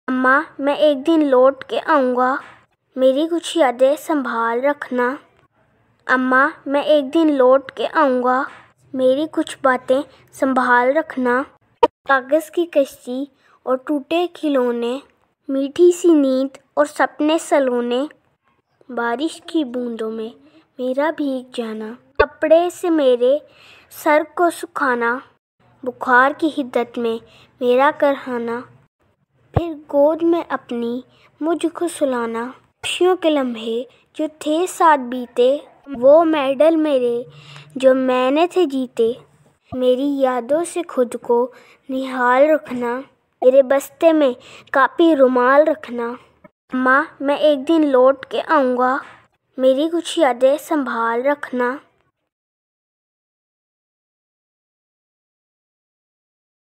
A tribute to APS Martyrs | 16-DEC-2014
In rememberanc of
APS Peshawar Attack on
May ALLAH reward them the highest place on Jannah and give patience to their relatives.

Poem written by Sajid Qamar
Voice over M. Muzammil Sajid
Pictorial data gathered from courteous websites. Thanks for their cooperation.
sajid qamar 11